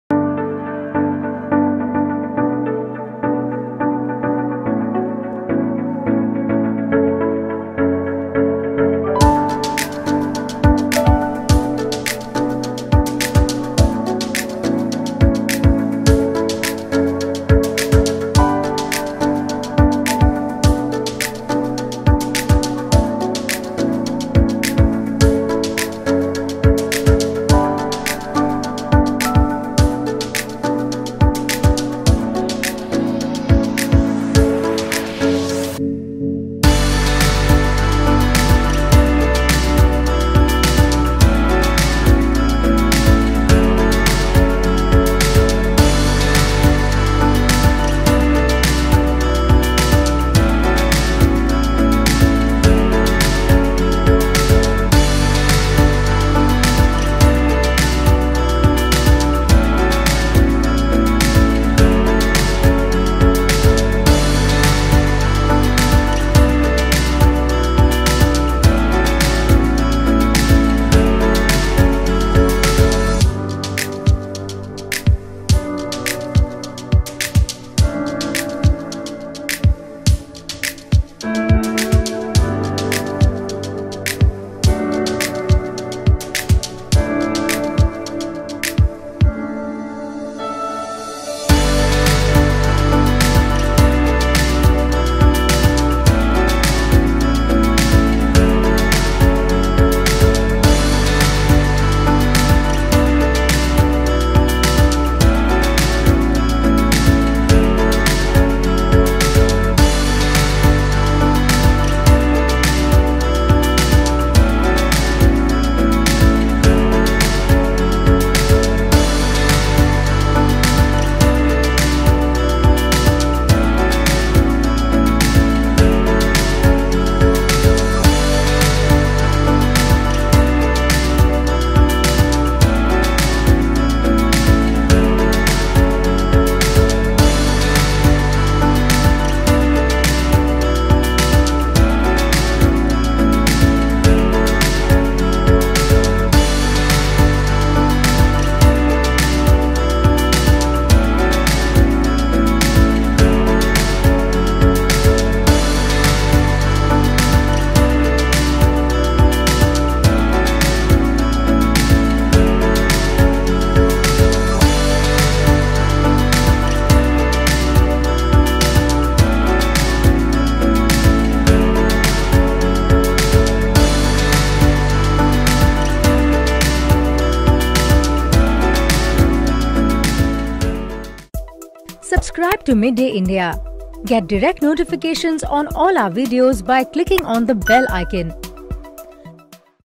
Central Railways 'Megh Veer' to rescue stranded local train during floods
The Central Railway has modified two WDS-6 class of diesel engines named MeghVeer which can work even when water is 12 inches above track level. These two engines will be helpful in pulling out localtrains stranded on the track during heavy flooding. Also, report on last month’s flooding, which left many trains stranded, recommends solutions to prevent waterlogging at Sion, Kurla, and Chunabhatti stations